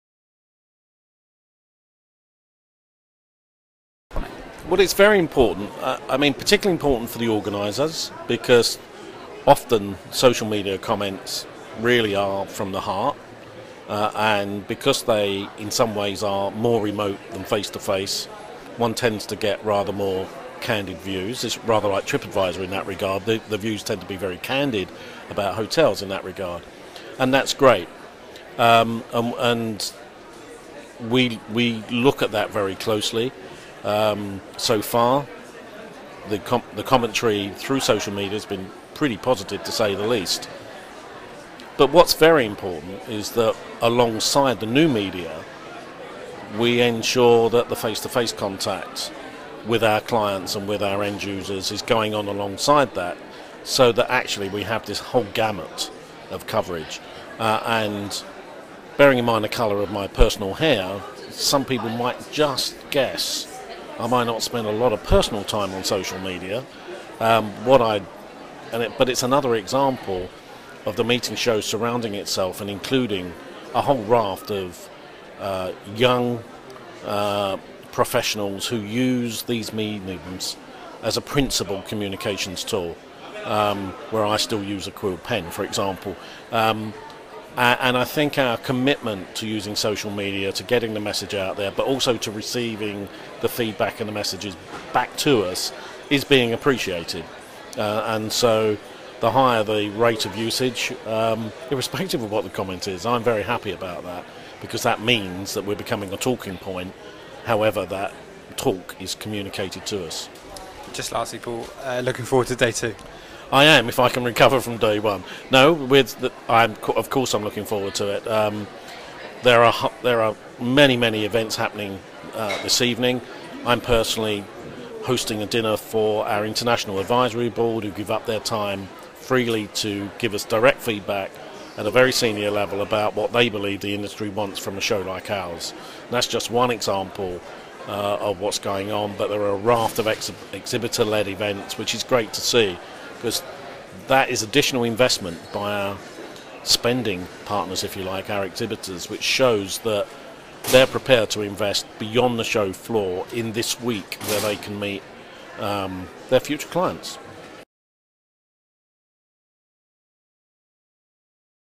Paul Kennedy on day one of The Meetings Show 2015
Paul Kennedy MBE looks back on day one of The Meetings Show 2015, the social media engagement and looks ahead to day 2.